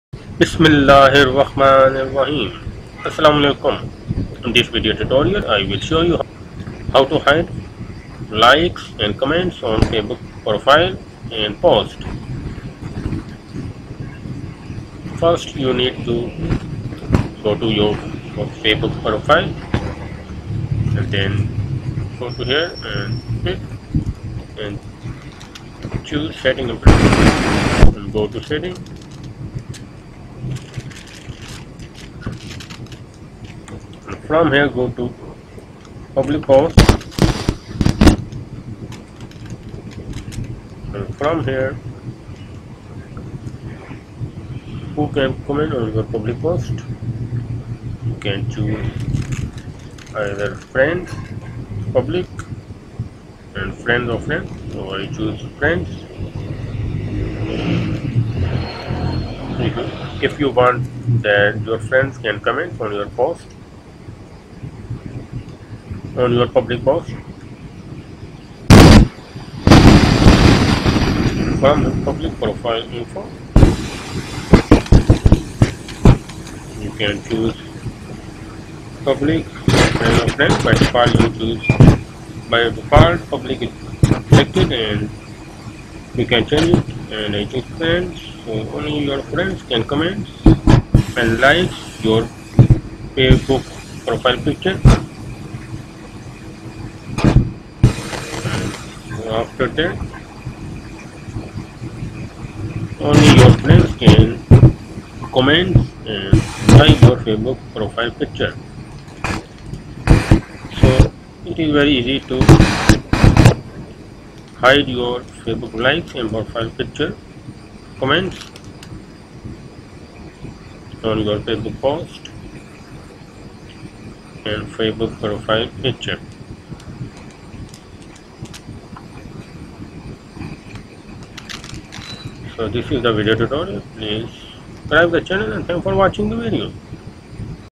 Hide Likes And Comments On Facebook Profile And Posts Easily
Hide your Facebook likes and comments on Facebook posts, Facebook profile picture, Facebook cover Photo, Timeline, etc. easily.
How To Hide Likes And Comments On Facebook Posts / Who Can Like Or Comment On Your Public Profile Pictures And Other Profile Info?:
Click account in the top right of Your Facebook Profile.

Select Settings & Privacy, then click Settings.
Click Public Posts.
Click Edit next to Public Profile Info.
Select who you'd like to comment on and like your public profile information: Public, Friends and Friends of Friends
You may Choose "Friends" so that only friends can like your posts.

How To Hide Comments On Your Public Posts / Who Can Comment On Your Public Posts
Click Public Posts.
Under Public Post Comments, click Edit and choose Friends or Friends of Friends

Who Is Allowed To Comment On Your Public Profile Pictures
Under Public Profile Info, click Edit and choose Friends or Friends of Friends to hide likes and comments from public.